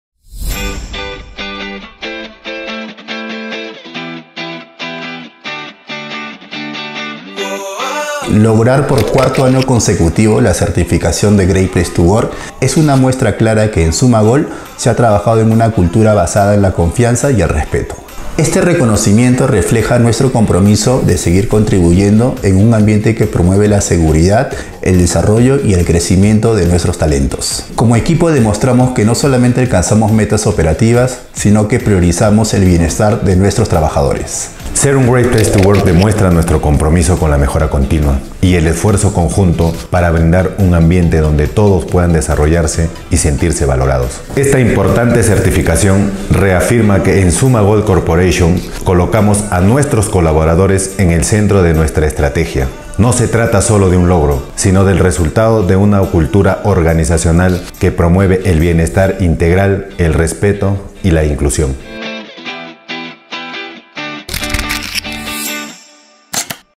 ORGULLO SUMMA || Certificación Great Place To Work 2024 - 2025
¡Cuarto año consecutivo siendo Great Place to Work! 🌟

En Summa Gold Corporation, hemos consolidado un entorno laboral donde la confianza, el respeto y el bienestar son nuestra prioridad. Este logro es un reconocimiento al compromiso de seguir creciendo juntos, cuidando el desarrollo y la seguridad de cada uno de nuestros colaboradores. En Summa Gold, ¡TODOS SOMOS CLIMA! 💪
Gerente General: Jaime Polar Paredes